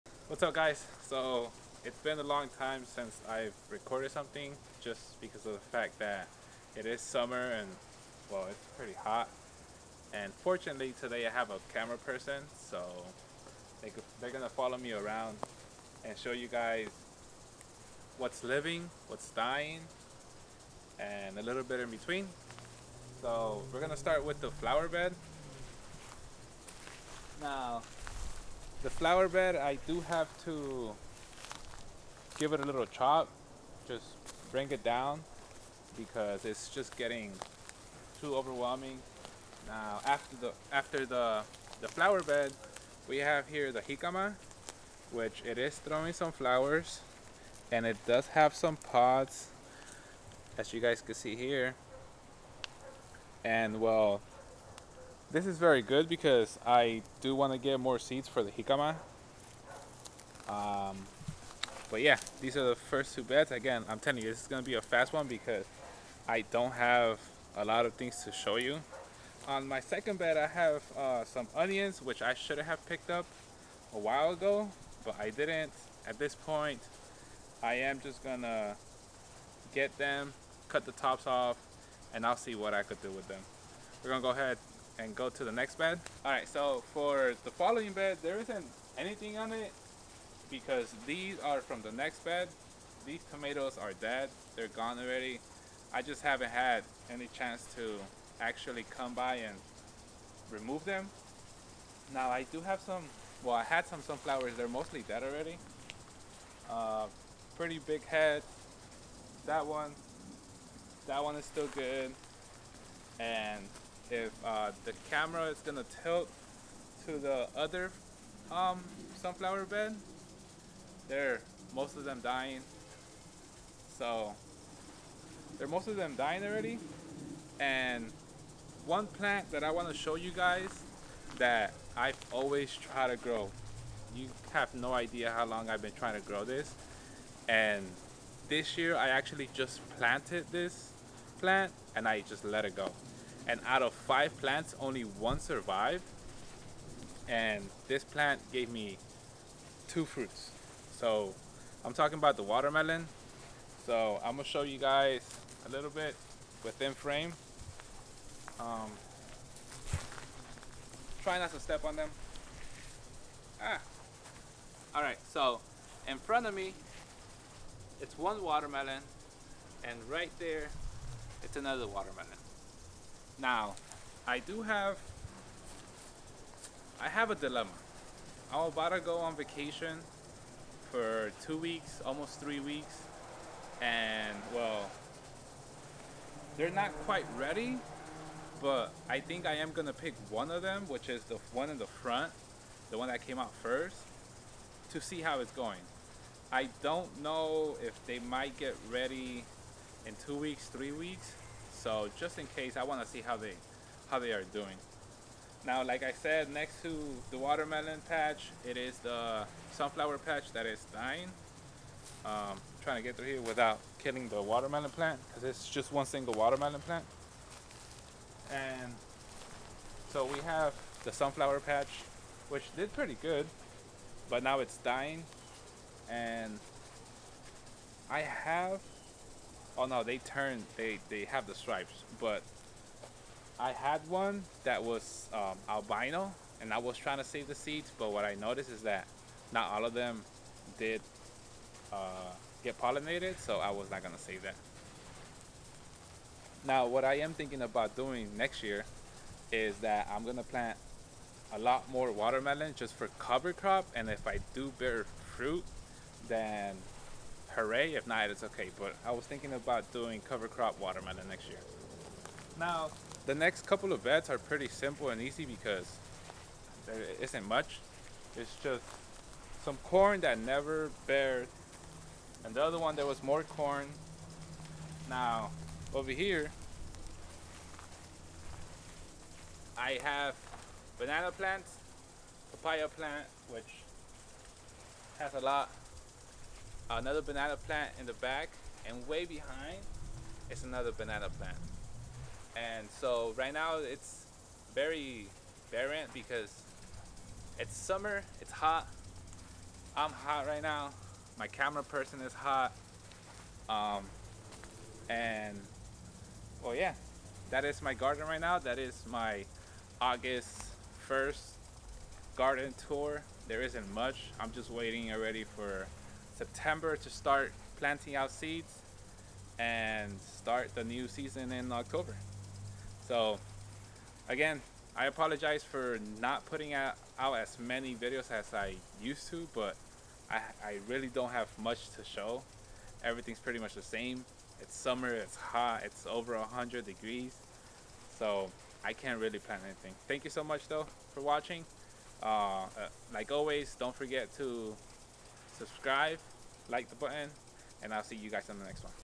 August garden update | South Florida gardening
Garden update on what's happening during summer in my garden.